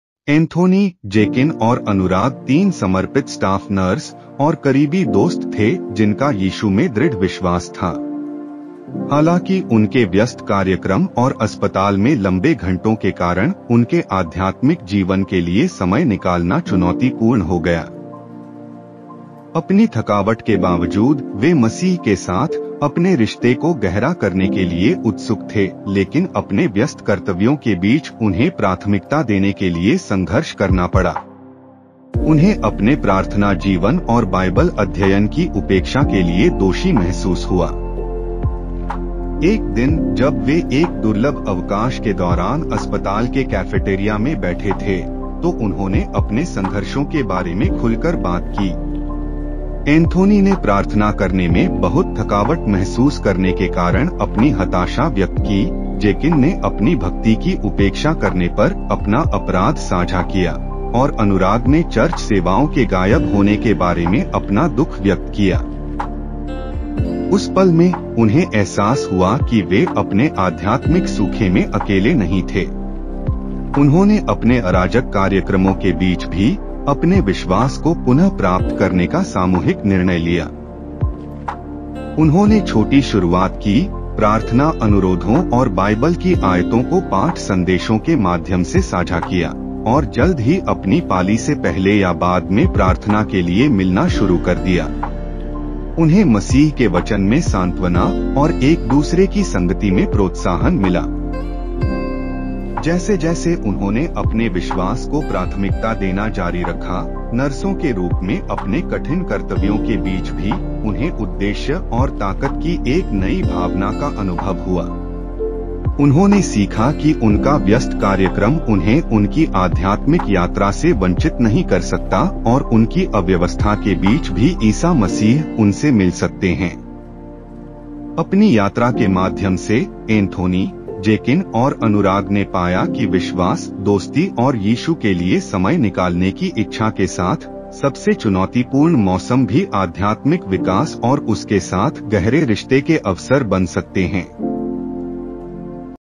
UNBREAKABLE BONDS : A STORY OF FAITH & FRIENDSHIP ll Audio/Visual Story - HINDI
Join us in this inspiring video as we follow the journey of three dedicated nurses, Anthony, Jekin, and Anurag, as they navigate the challenges of balancing their demanding schedules with their desire to deepen their relationship with God. Discover how they found strength, purpose, and friendship in the midst of chaos.

Key Takeaways:

Prioritizing faith is essential even in busy times.

Community support and accountability play a significant role in spiritual growth.

Making small changes can lead to significant transformations in one's spiritual life.

Step by Step Process:

Open Up and Share Struggles: Confess frustrations and guilt about neglecting spiritual practices.

Make a Collective Decision: Commit to reclaiming faith despite hectic schedules.

Start Small: Share prayer requests, verses, and commit to meeting for prayers.

Find Solace in God's Word: Seek encouragement and support from each other.

Experience Renewal: Witness the transformative power of prioritizing faith in your daily life

Personal Advice: Always remember that even in the busiest of times, making time for your spiritual well-being can lead to a sense of peace, purpose, and strength amidst chaos.